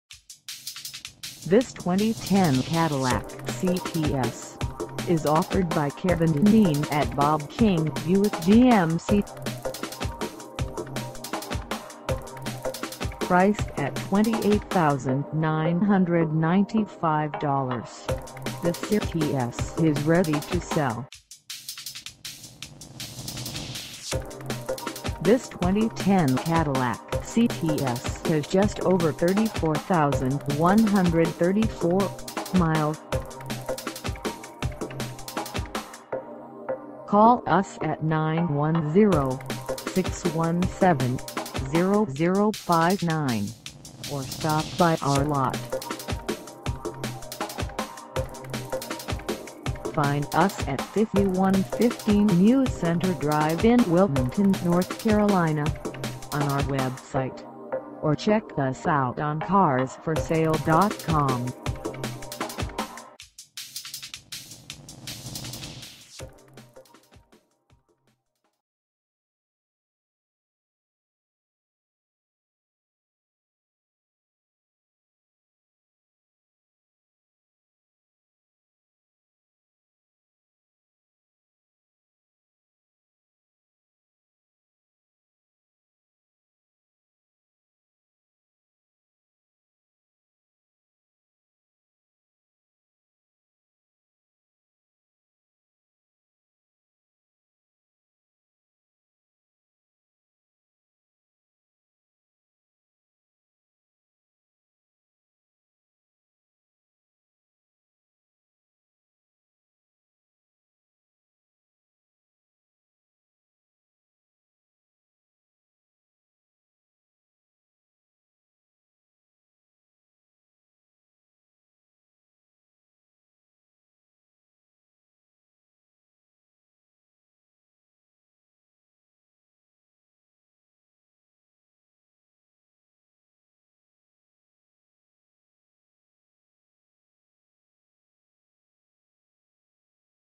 2010 Cadillac CTS Performance - for sale in Wilmington, NC 28403
Kevin Dineen at Bob King Buick GMC
5115 NEW CENTRE DR

Come test dirve this 2010 Cadillac CTS Performance for sale in Wilmington, NC.

Power UltraView Double-Sized Sunroof. Yeah baby! Yes! Yes! Yes! You won't find a better wagon than this charming 2010 Cadillac CTS. It's the combination of advanced design and precision engineering that gives the CTS the pinned-to-your-seat acceleration. New Car Test Drive said its''...dynamics, handling and isolation are all very, very good. The level of luxury and the quality of build, fit and finish is very high, and the pure, edgy style of this car is breathtaking...'' Designated by Consumer Guide as a 2010 Premium Midsize Car Best Buy.